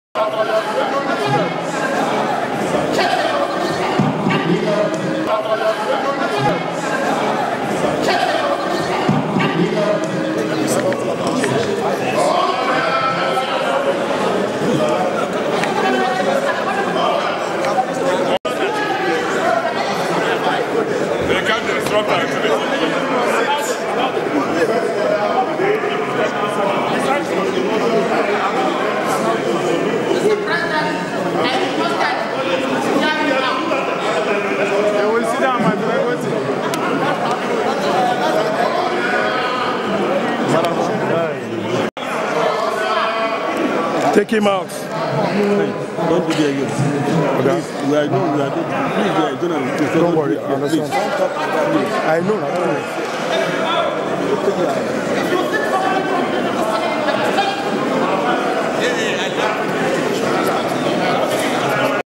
BIAFRA;- Senator Ike Ekweremadu Blow Hot Air on Pres. Buhari over Unlawful Arrest of Innoson by EFCC
u Blow Hot Air on pres. Buhari over Unlawful Arrest of Innoson by EFCC